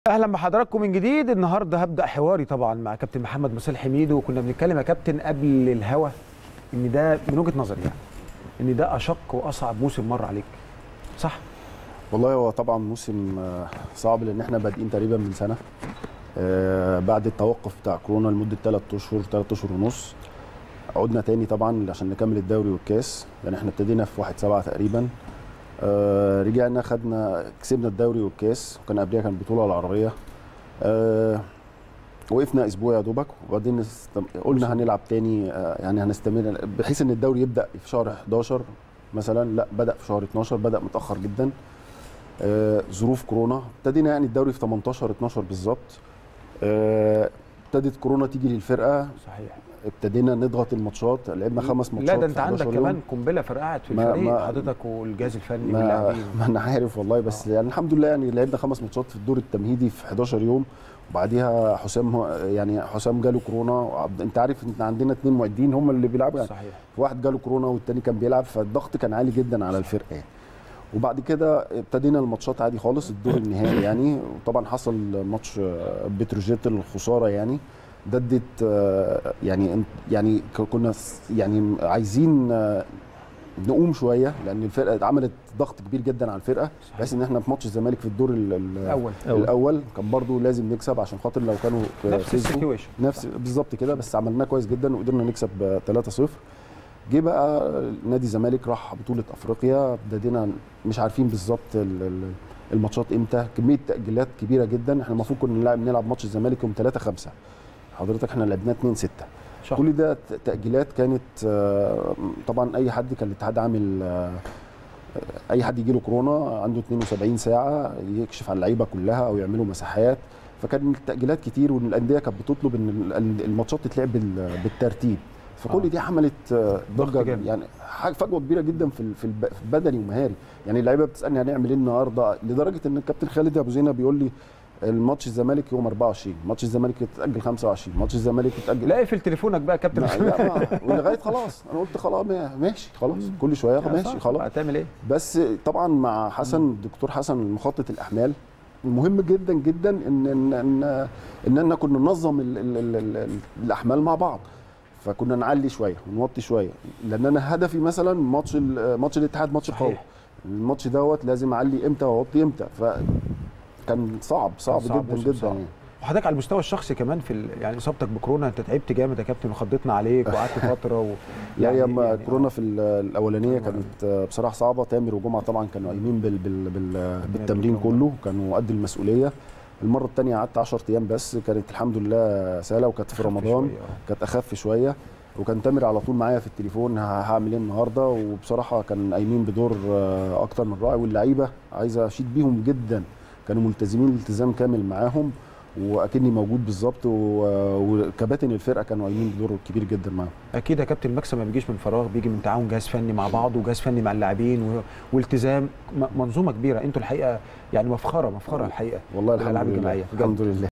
محمد مصيلحي: من اصعب المواسم بسبب كثرة التوقف وغياب العديد من اللاعبين للاصابة بفيروس كورونا.
محمد مصيلحي: من اصعب المواسم بسبب كثرة التوقف وغياب العديد من اللاعبين للاصابة بفيروس كورونا | الابطال